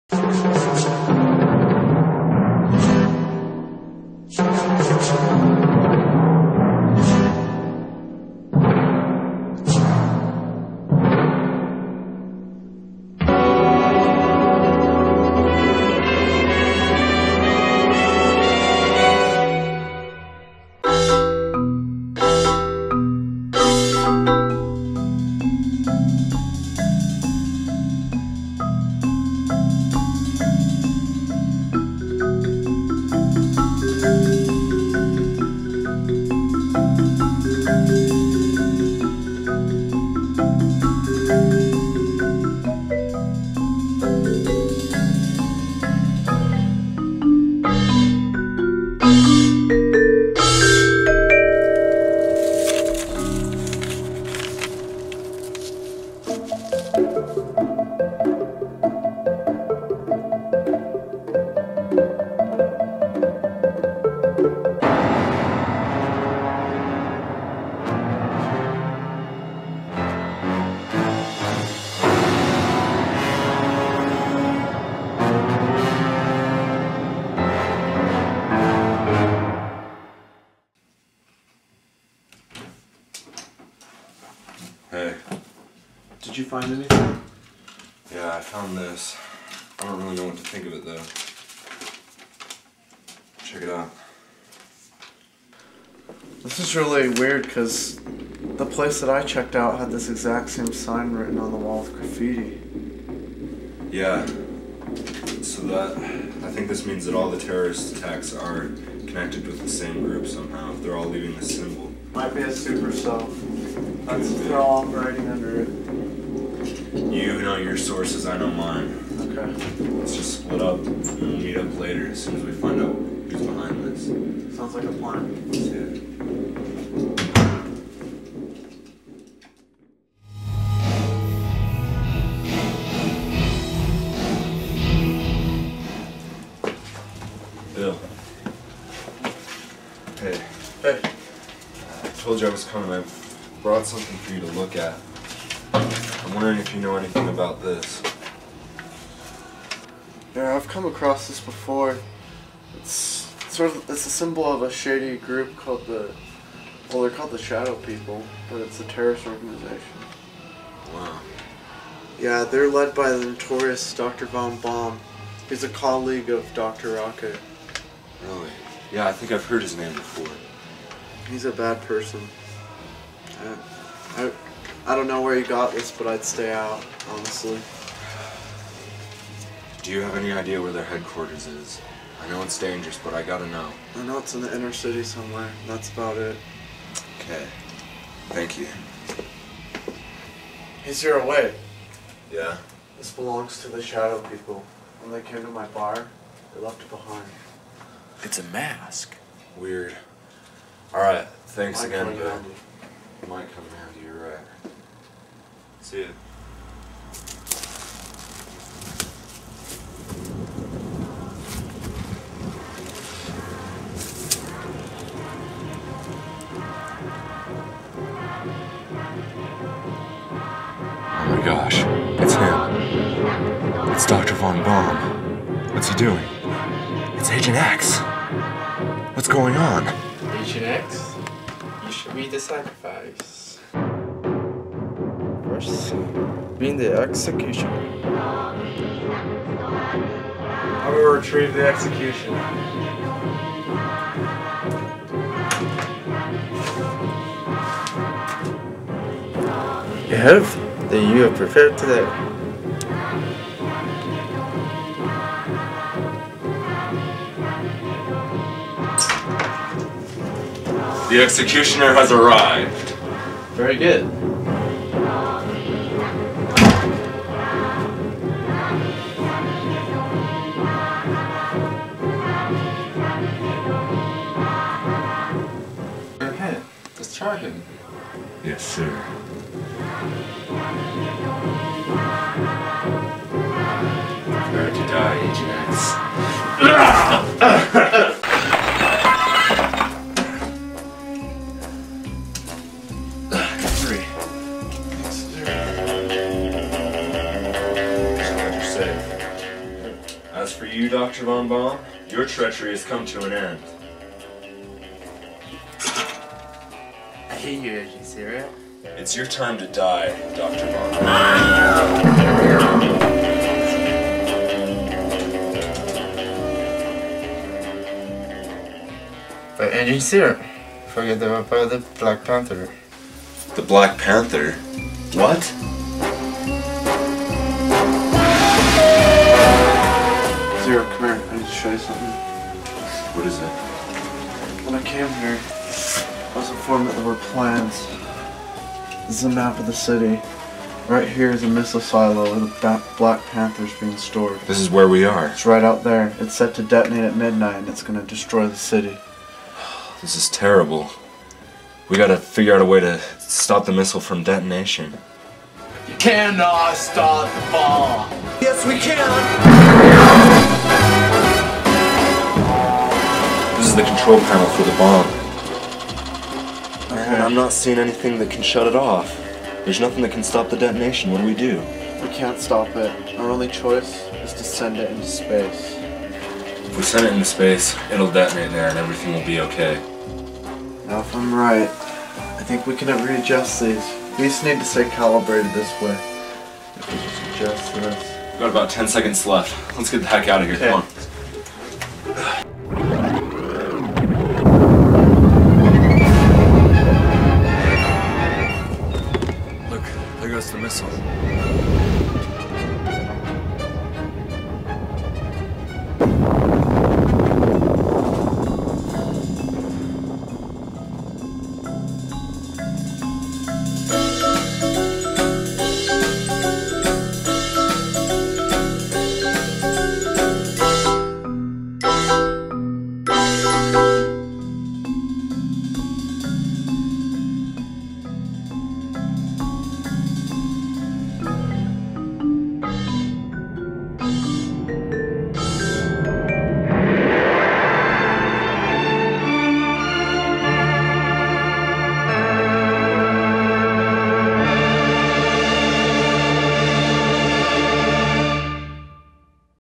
Agent Zero and The Shadow People
Zero and X hunt down a cult of terrorists known as the Shadow People.

FUN FACT! Roberto Ahumada's brilliant performance as Doctor Von Bomb was inspired by his first time viewing of Dr. Strangelove.

2006, Curtis Robert Video